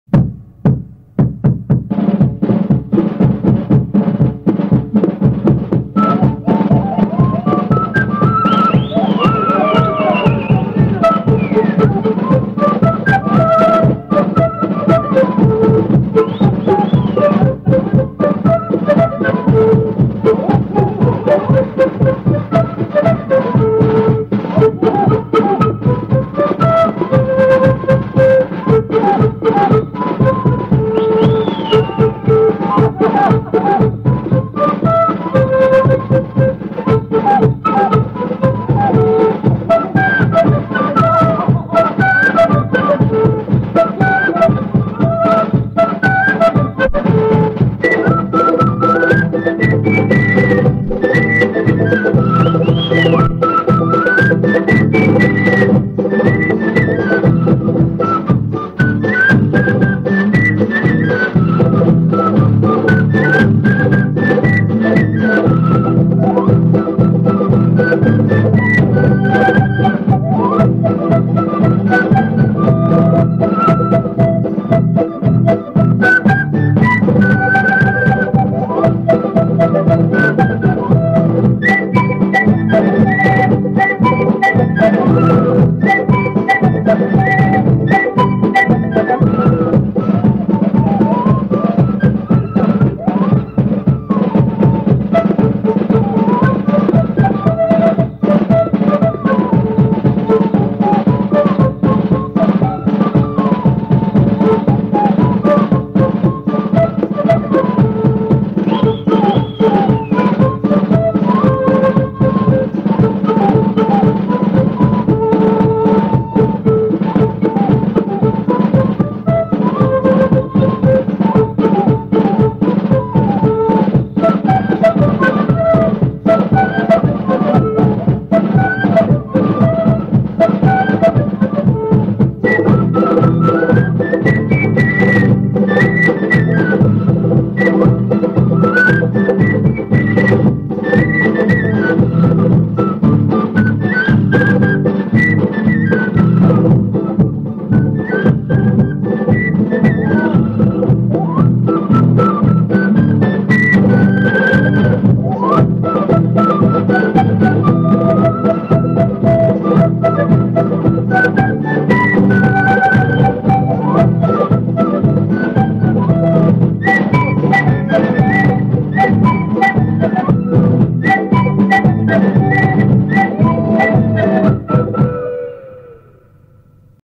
GRUPO QUINTA - MORENADA - DEL FOLCLORE
Rescate de un trabajo musical de un cassette del grupo Quinta.